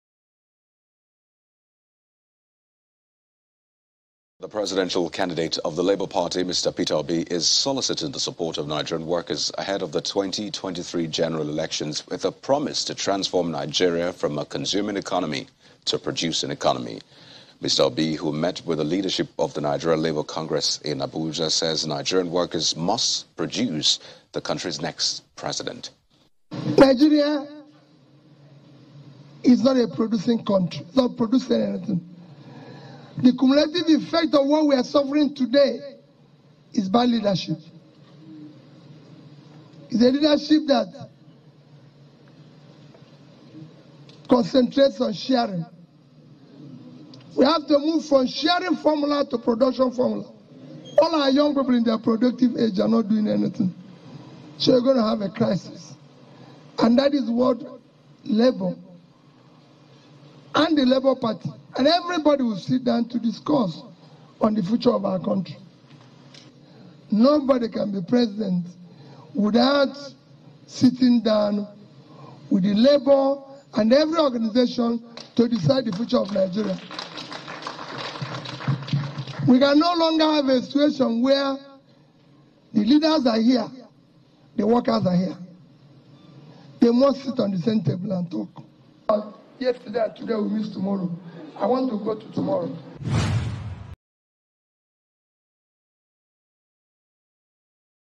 Nigeria Not A Producing Country – Peter Obi
The presidential candidate of the Labour Party, Peter Obi, on Tuesday, said Nigeria is suffering from bad leadership as she is not a producing country.

Obi stated this during a visit to the leadership of the Nigerian Labour Congress (NLC) and the Trade Union Congress (TUC) in Abuja, the nation’s capital.

He stated that the citizens are not receiving the best from the ruling class, calling for a change from the sharing formula to the production formula.